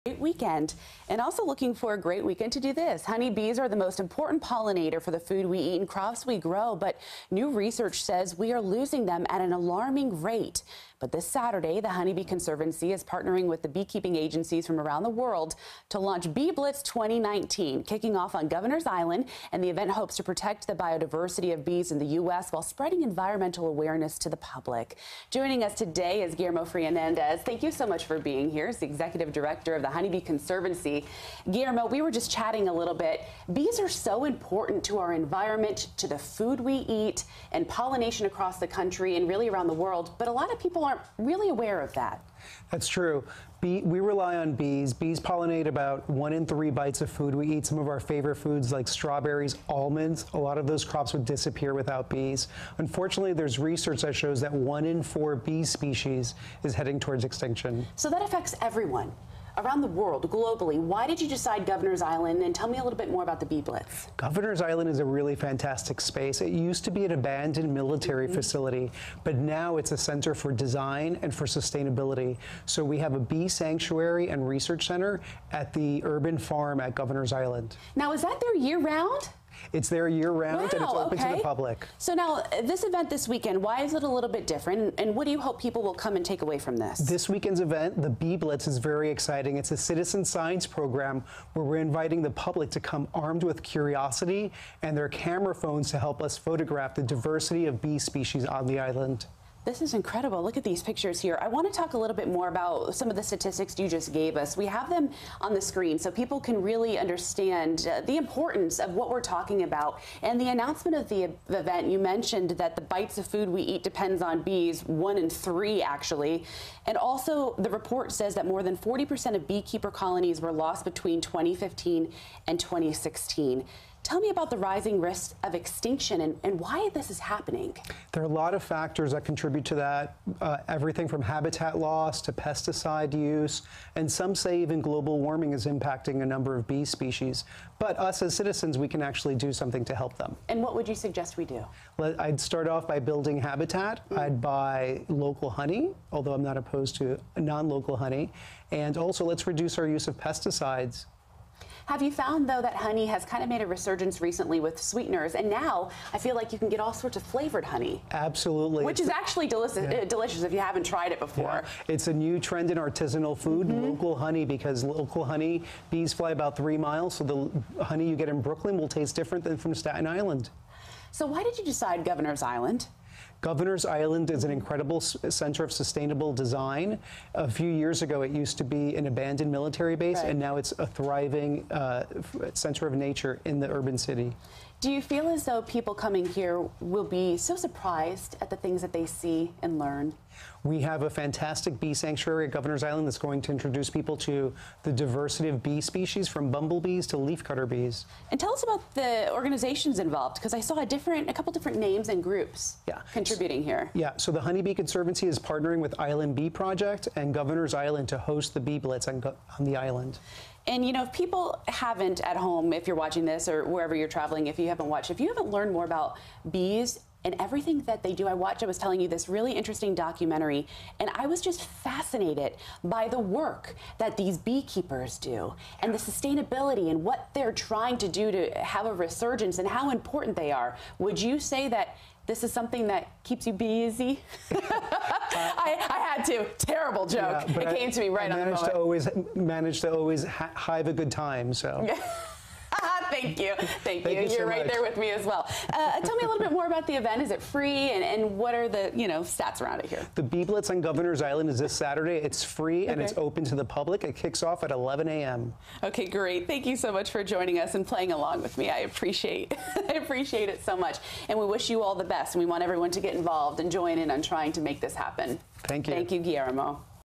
Bee Blitz 2019 Kicks Off On Governors Island
Honey bees are the most important pollinator for the food we eat and crops we grow, but new research says we're losing them at an alarming rate. This Saturday, the Honey Bee Conservancy is partnering with beekeeping agencies from around the world to launch Bee Blitz 2019.